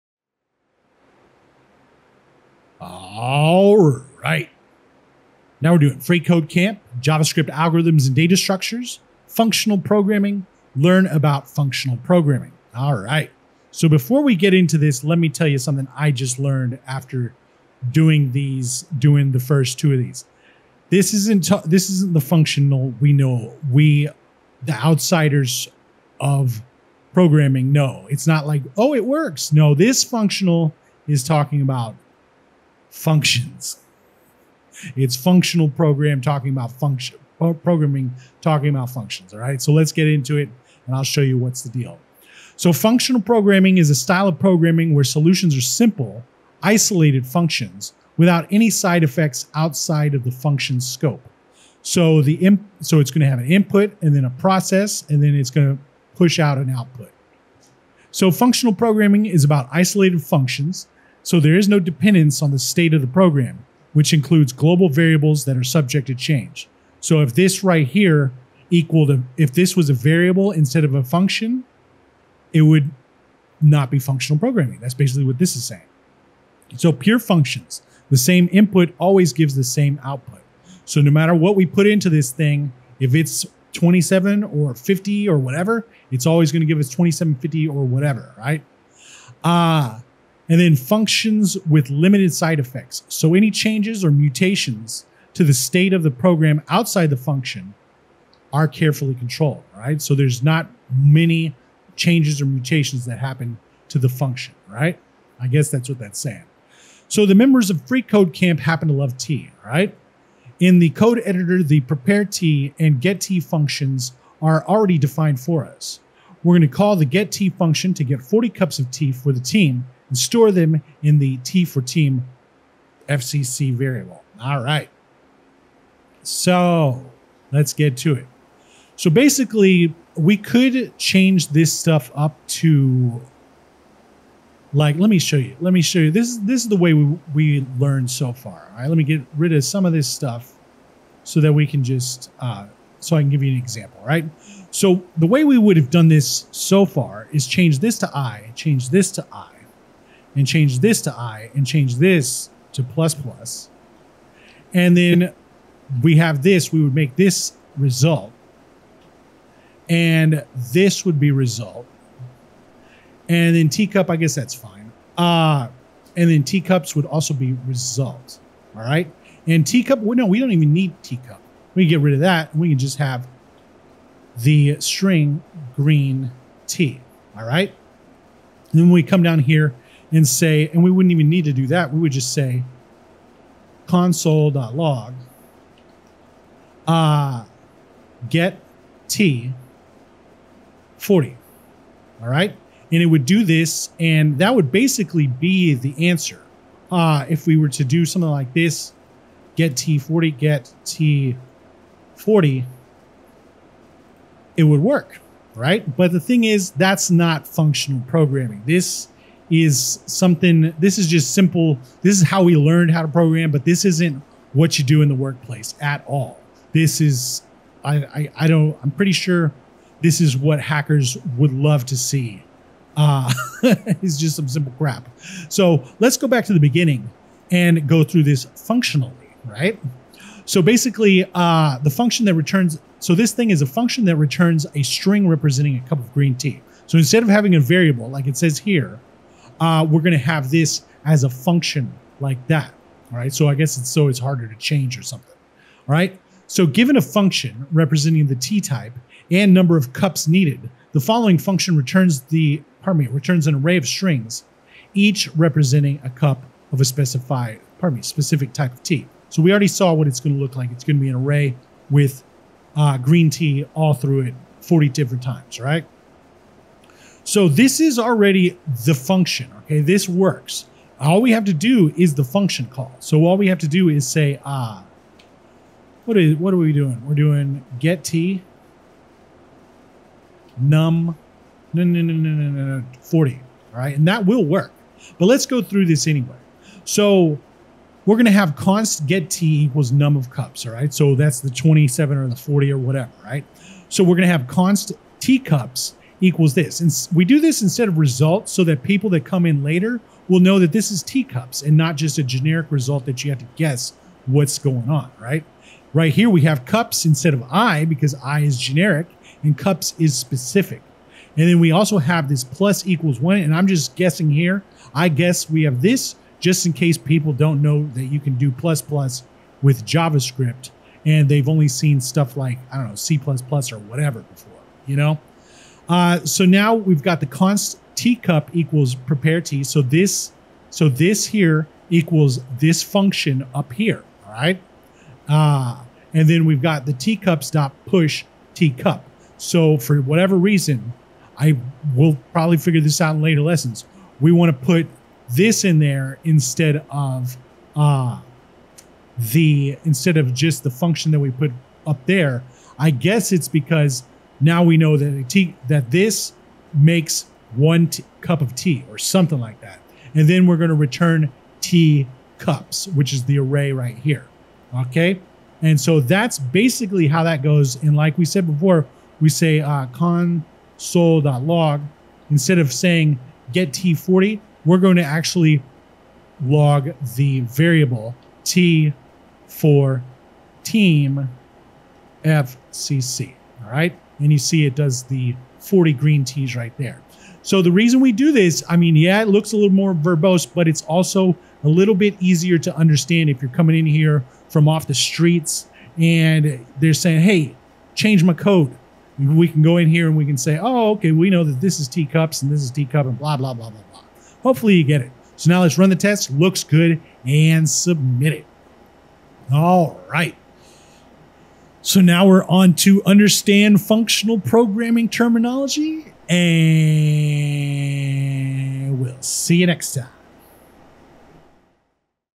Learn About Functional Programming | freeCodeCamp
🌿 **"Sipping on Simplicity: The Essence of Functional Programming"** 🌿

In today's digital age, we seek solace in the clarity of simplicity, and what better way to find it than through the elegance of Functional Programming. Join us in a serene exploration of programming where each function is a self-contained whisper of logic, stirring the vast ocean of code with the gentlest of waves.

🌐 **A Journey Through Functionality**:

Embark on a tranquil quest with us as we embrace the principles of Functional Programming. Here, every function is an island, serene and untouched by the tumultuous seas of global state.

🍃 **The Purity of Purpose**:

With each function crafted like a haiku, we celebrate the purity of purpose in our code. Discover with us how the same serene inputs always result in the same predictable outputs, as reliable as the sunrise.

🌼 **The Art of Isolation**:

Marvel at the art of isolated functions, where dependencies disperse like morning mist, and the function's soul remains untainted by the mutable world outside.

🍵 **The Essence of Tea**:

Watch as we demonstrate these principles through the simple way of preparing tea, a metaphor for the nourishing INPUT / PROCESS / OUTPUT pattern that is the heart of Functional Programming.

🫖 **Steeping the Perfect Cup**:

Follow the preparation of 'tea4TeamFCC', not just a variable but a vessel, holding 40 cups of meticulously crafted tea, each an ode to the disciplined beauty of code without side effects.

✨ **Elevate Your Coding Zen**:

Share in the joy of a discipline that transcends the code editor and seeps into the calmness of a coder's mind.

🌟 **Hashtags to Connect With**:

Through the quietude of Functional Programming, we find a retreat from the complexities of mutable states and side effects. Let's steep ourselves in this mindful approach, where every function we write is like preparing a perfect cup of tea, simple and serene.